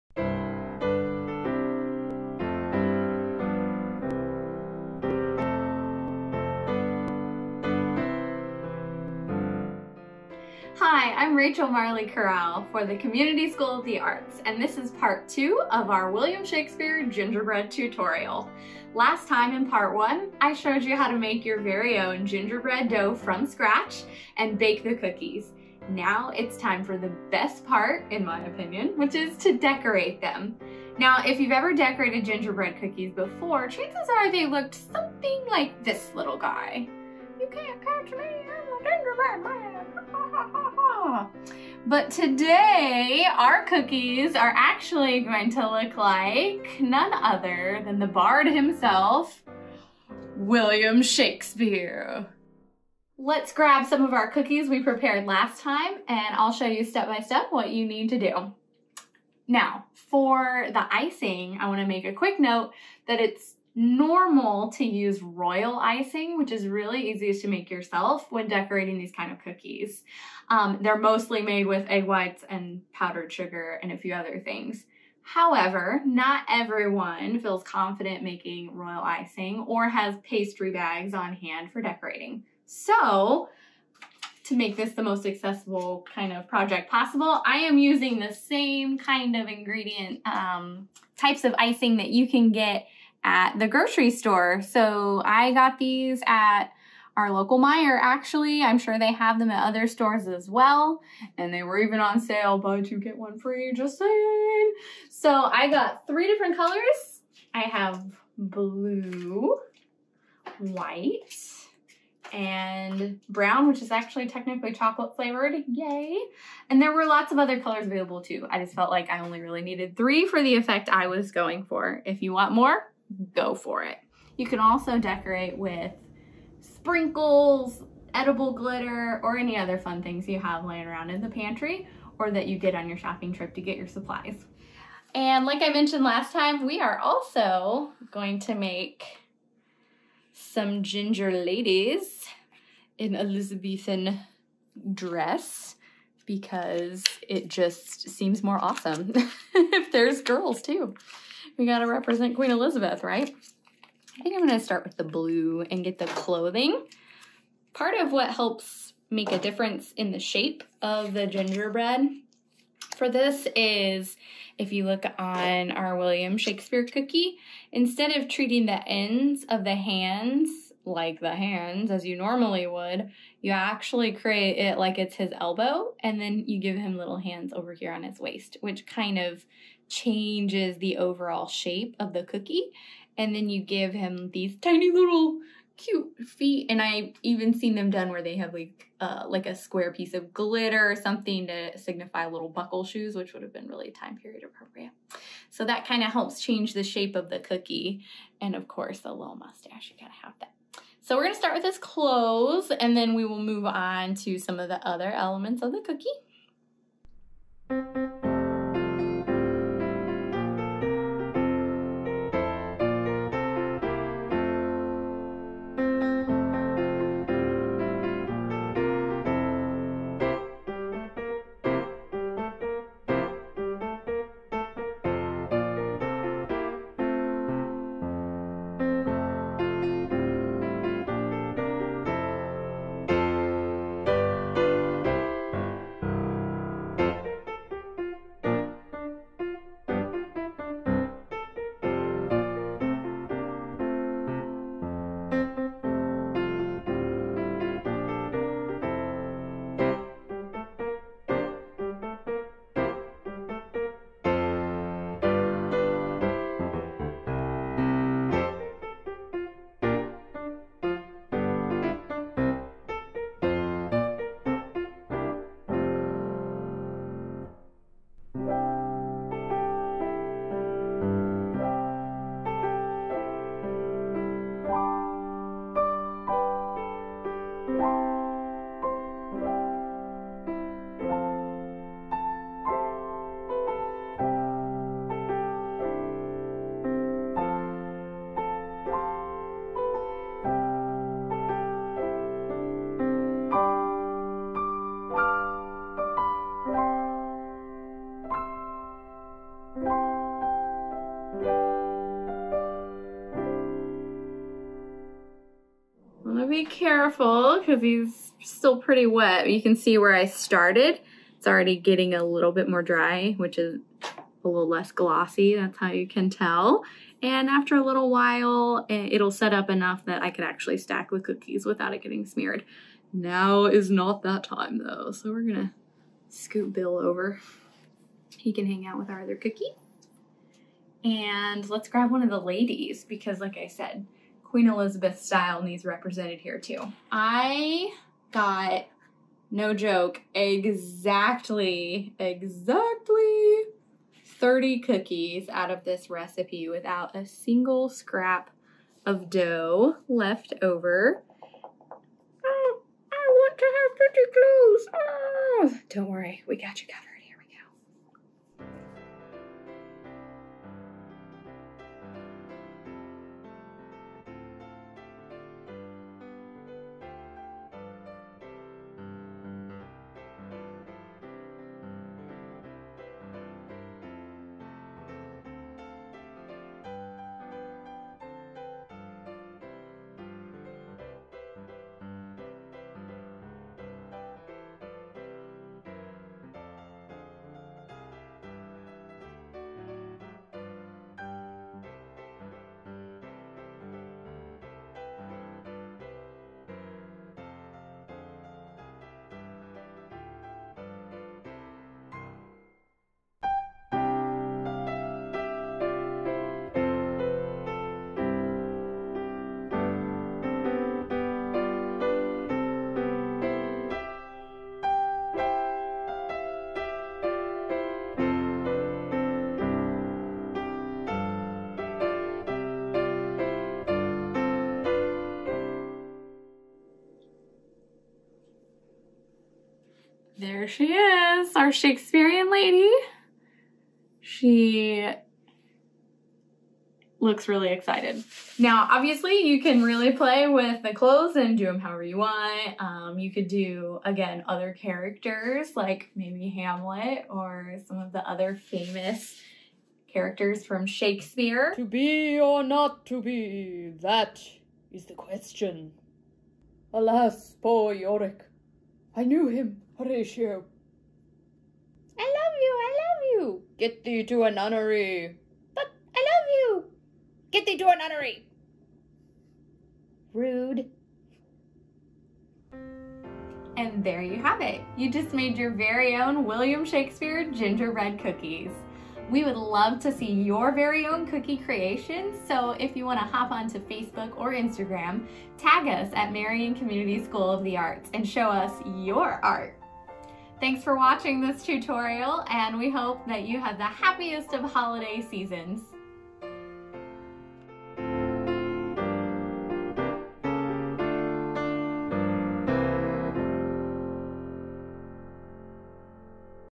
Shakespeare Cookie Tutorial: Part 2 (Decorating)
Join CSA Director of Theatre Rachel Marley Corral as she teaches you how to make your very own gingerbread cookies from scratch... that also happen to look like William Shakespeare! The Bard has never looked quite so delicious! ;)

Follow the CSA Theatre compANY on Instagram!